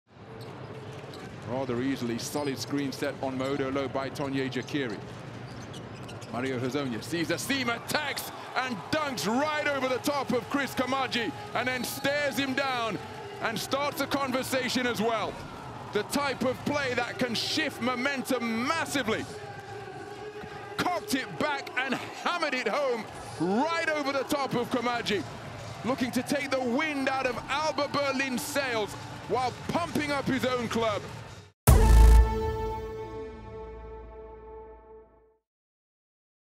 7DAYS Magic Moment of the Night: Mario Hezonja posterizes Koumadje!
Mario Hezonja drives and then posterizes the giant Christ Koumadje.